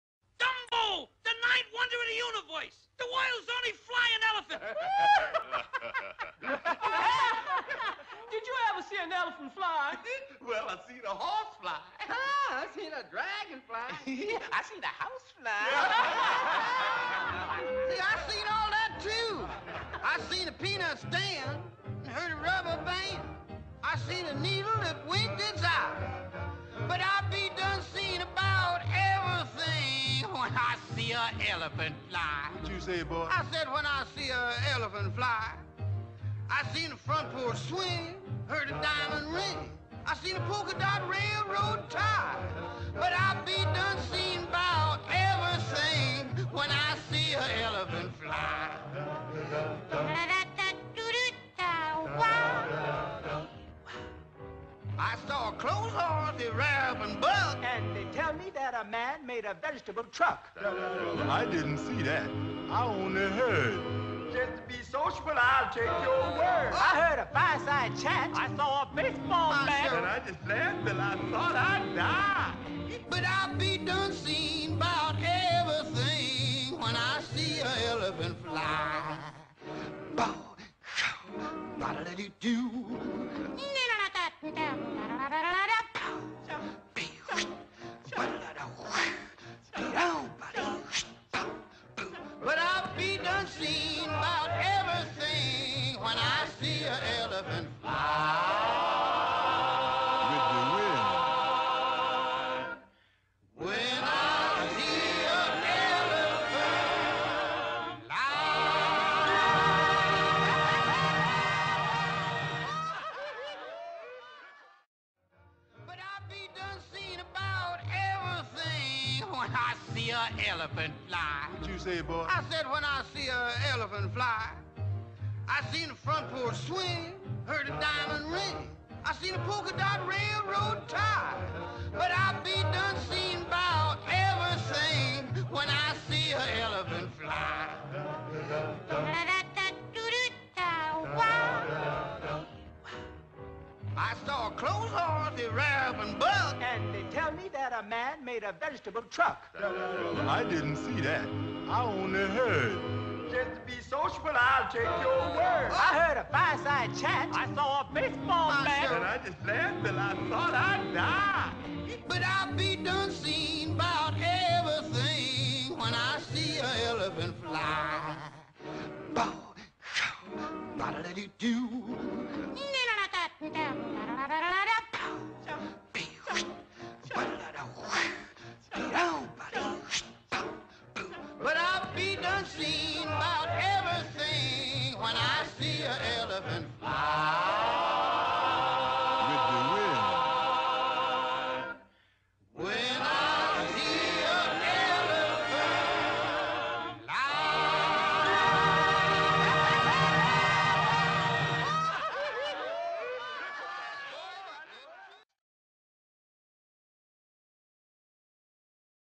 Dumbo The Elephant Speed Art on Paint
Back in the game!! with a quick speed art for you guys :) dumbo the elephant, took me about 2 hours and was alll done on microsoft paint using only a mouse

Plaease leave a comment of what you want to see next and share it around for all your friends to see!!!

Thanks :)